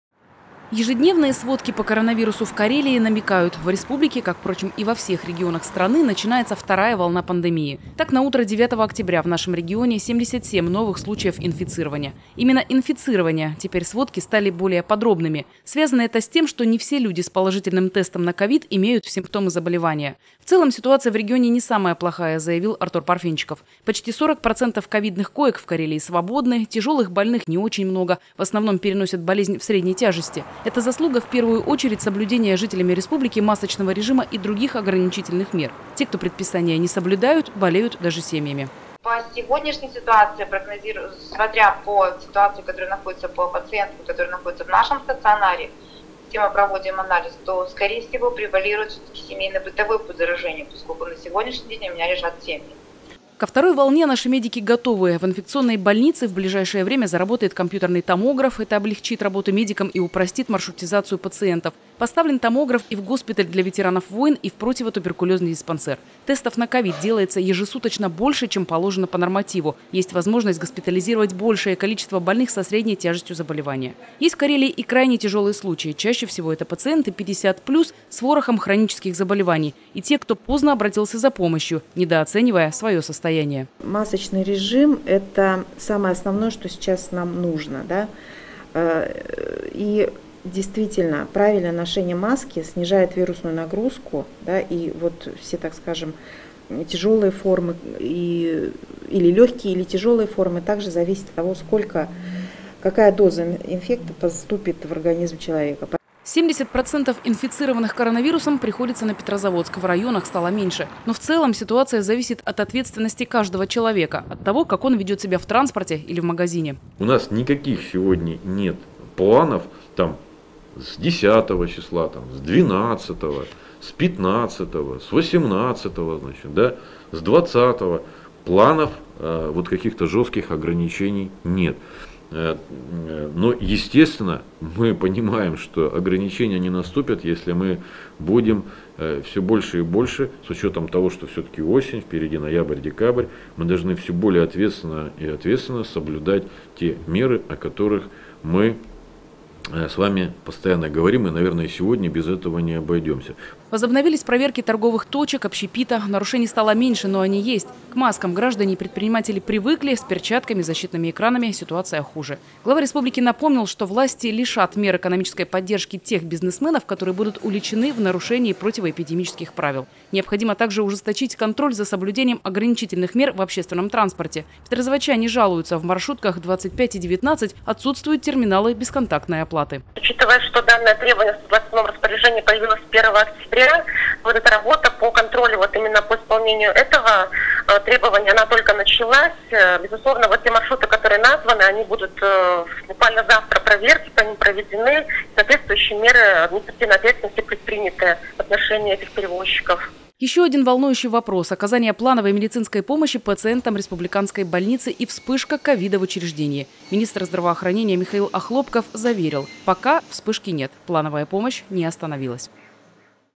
Прямая линия Главы Карелии: ситуация с распространением в регионе коронавирусной инфекции.
"В Карелии всё может закрыться на карантин в любой день, но наша главная задача - сделать так, чтобы этого не произошло."(С)

Так заявил Глава республики в ходе Прямой линии в сети ВКонтакте, которая была посвящена ситуации с распространением в регионе коронавирусной инфекции. В трансляции приняли участие руководители Министерства здравоохранения и Роспотребнадзора, представители инфекционной больницы и Госпиталя для ветеранов войн (ныне - ковид-центра).